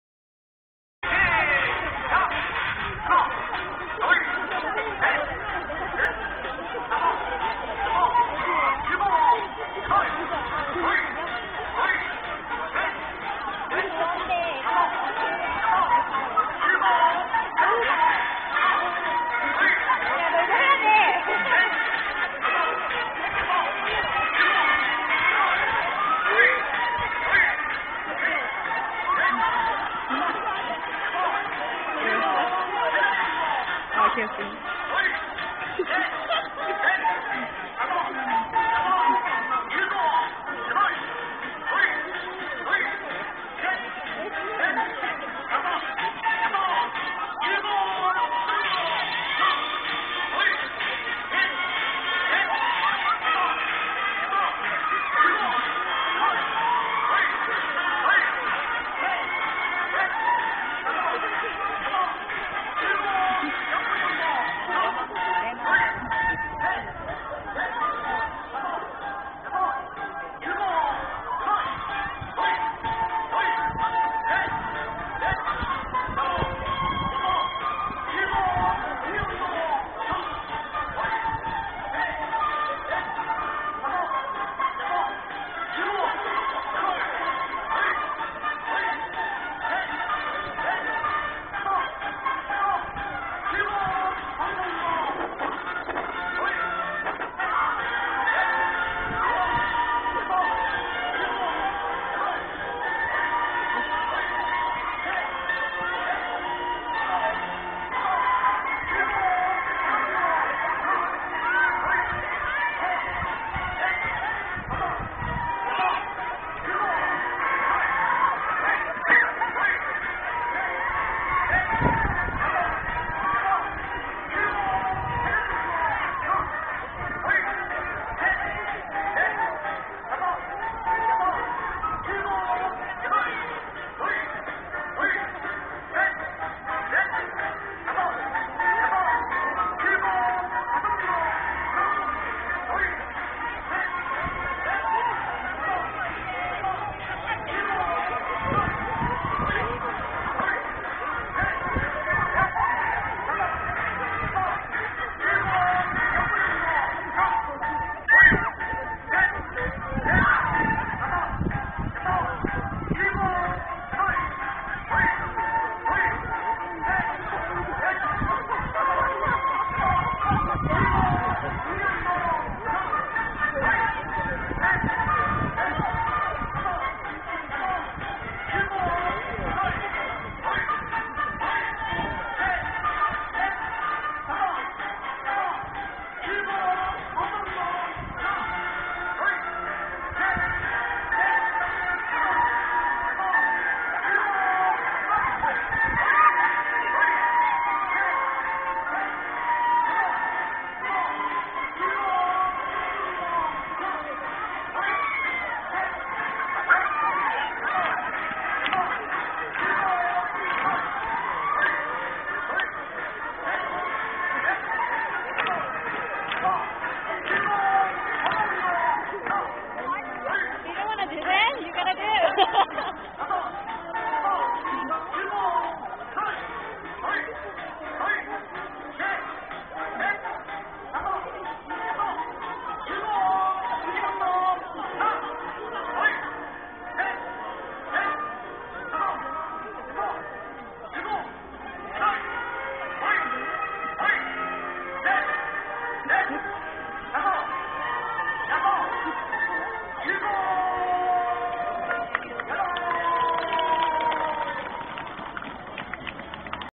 Sports Day 2011 at CJGMS
The warm up at my middle school during Sports Day in South Korea. For more information on sports day, check my blog post